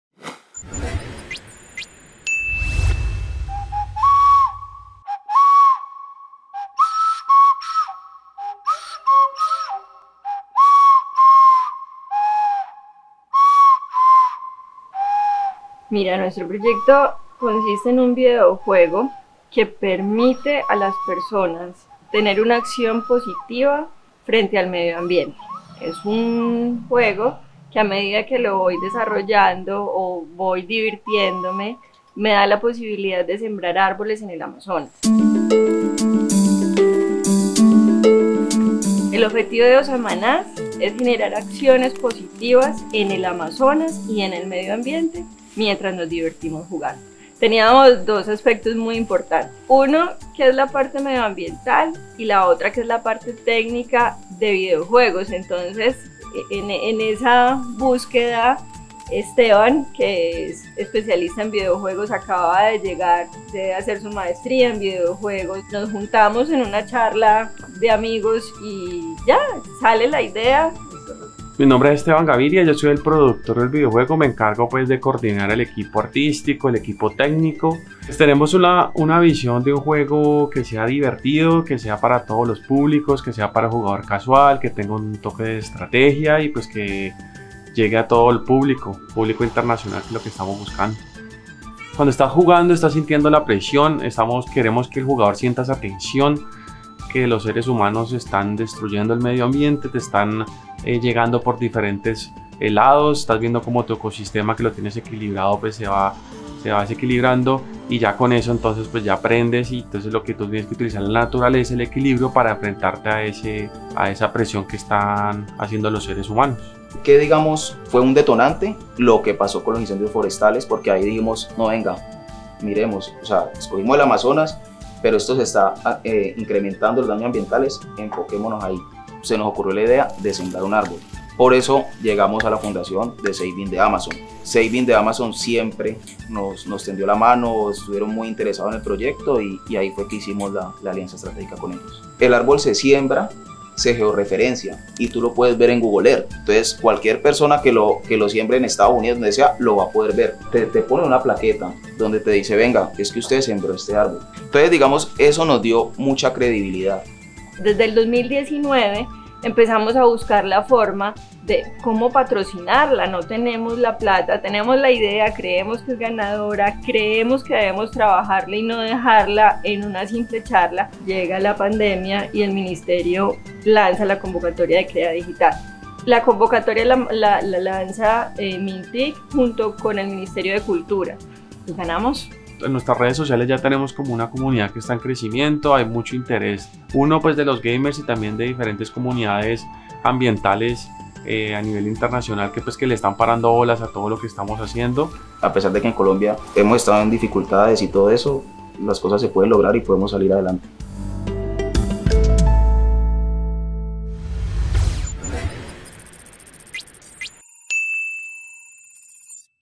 Conoce a Jaime Andrés Gonzalez beneficiario de InternetParaLaVida
La pasión por los videojuegos y las ganas de contribuir con la reforestación del Amazonas tras el gigantesco incendio que ocurrió en 2019, fueron la motivación de Jaime Andrés para crear “AMAZONIAN THE LAST HOPE”, Un videojuego que trasciende a la vida real. Los jugadores deben completar misiones y al final pueden elegir que árbol sembrar. En Amazonas la asociación Saving The Amazon junto con los indígenas se encargan de la siembra y enviarle al jugador fotos y georreferenciación de su árbol.